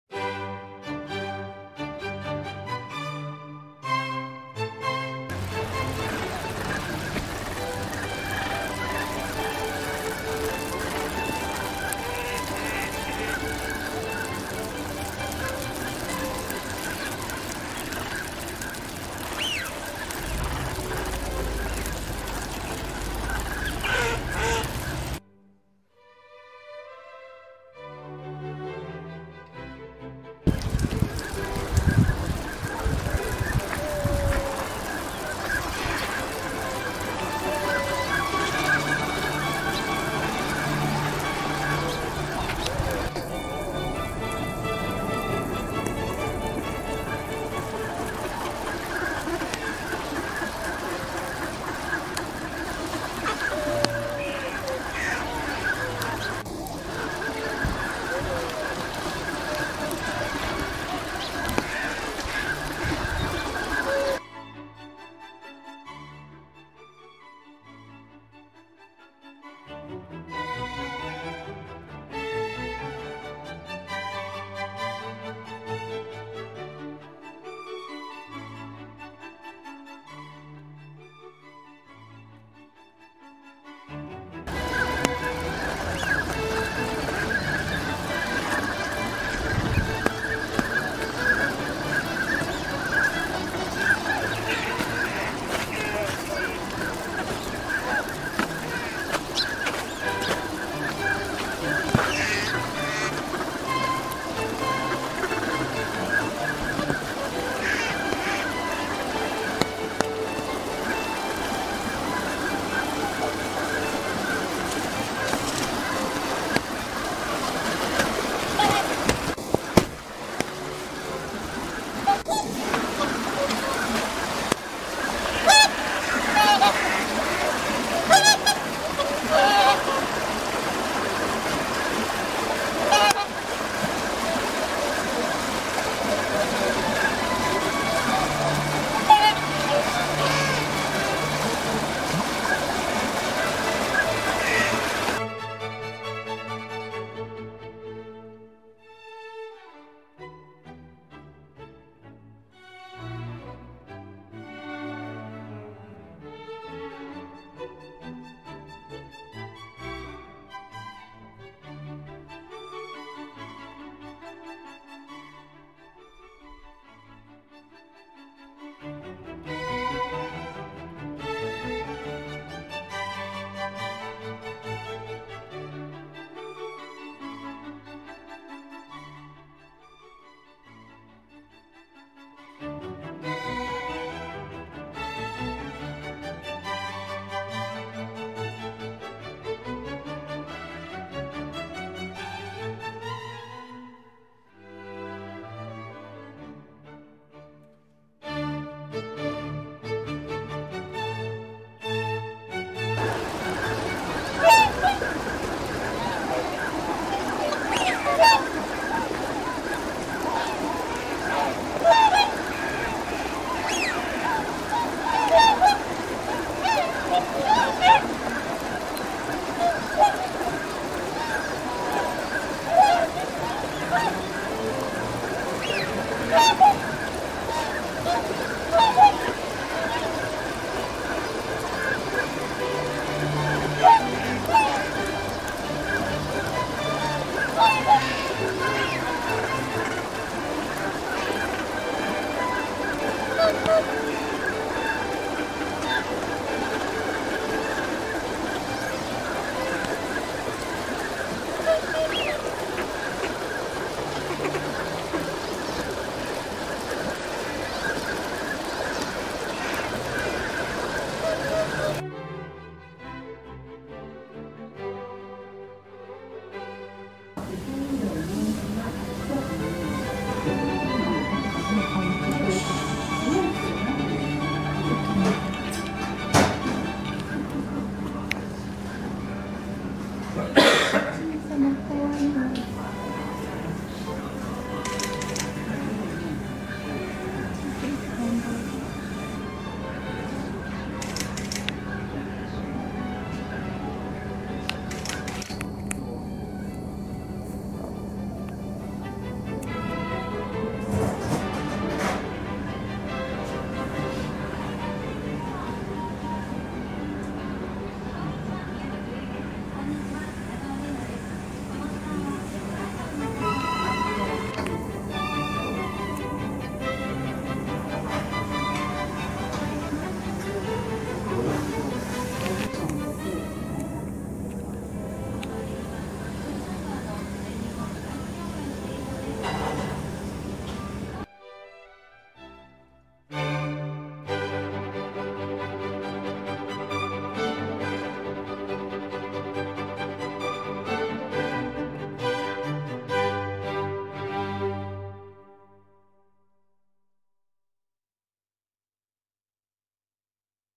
水原「瓢湖」の白鳥たち：Part.Ⅱ
この日は雨のち雪。白鳥は約2,000羽いる筈？、鴨たちに圧倒され今日は100羽ほどである。